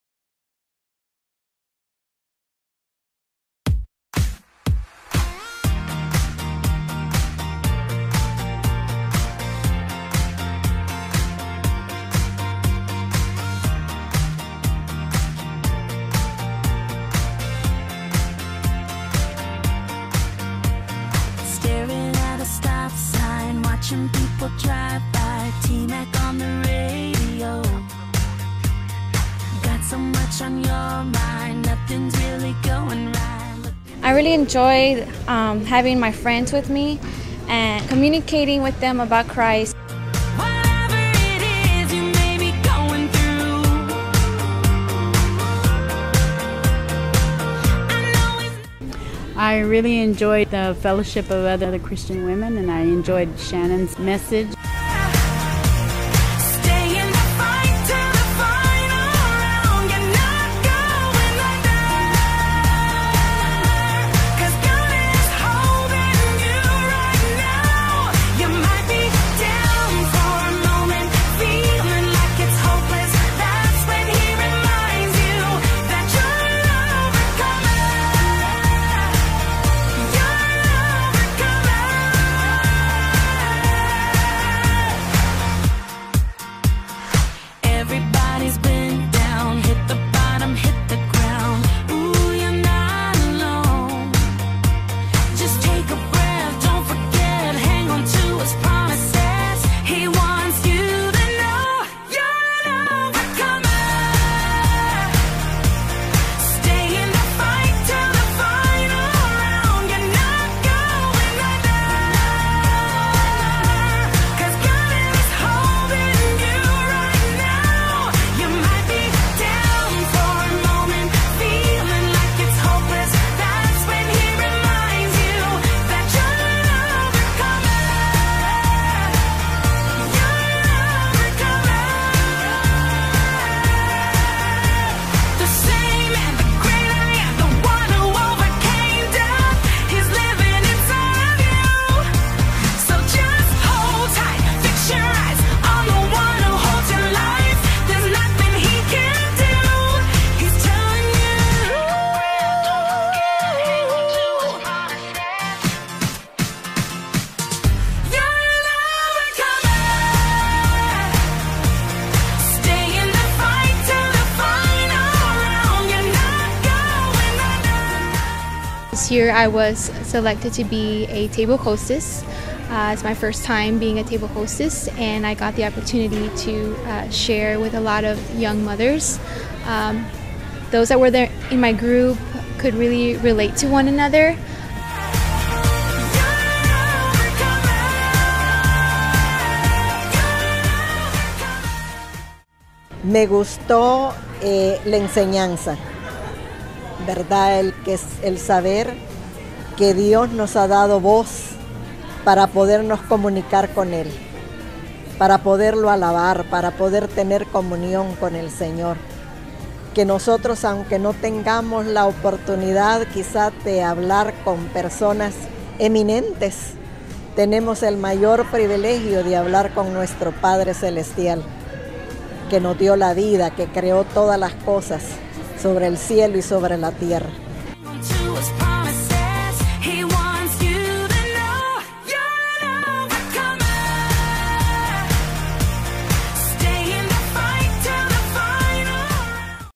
Houston Women of Heart Retreat 2014
Corazón Ministries presents: Women of Heart Retreat 2014 ''Fashioned by God's Design''. Houston, TX.